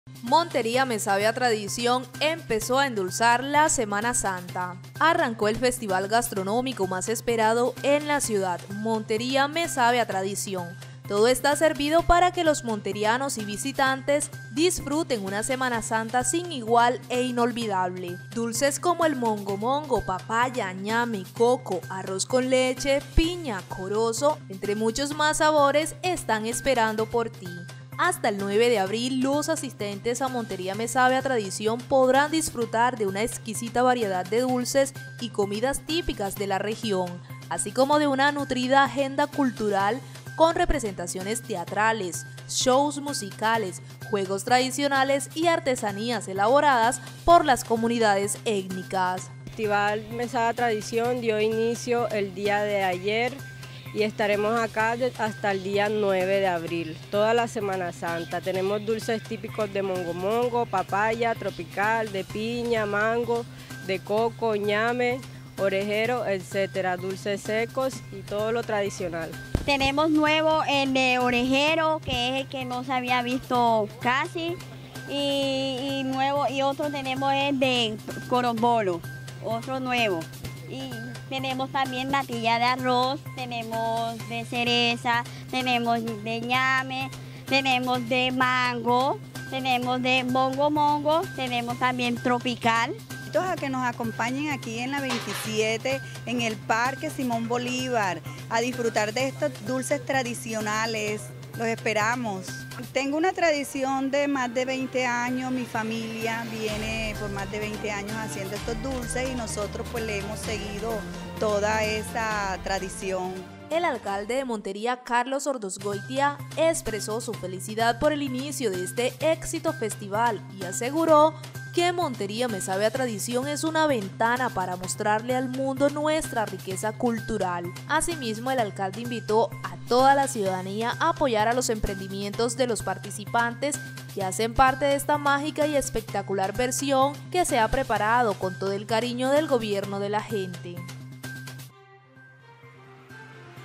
Montería Me Sabe a Tradición” empezó a endulzar la Semana Santa
Arrancó el Festival Gastronómico más esperado en la ciudad, Montería Me Sabe a Tradición. Todo está servido para que los monterianos y visitantes disfruten una Semana Santa sin igual e inolvidable. Dulces como el Mongo Mongo, Papaya, Ñame, Coco, Arroz con Leche, Piña, Corozo, entre muchos más sabores, están esperando por ti.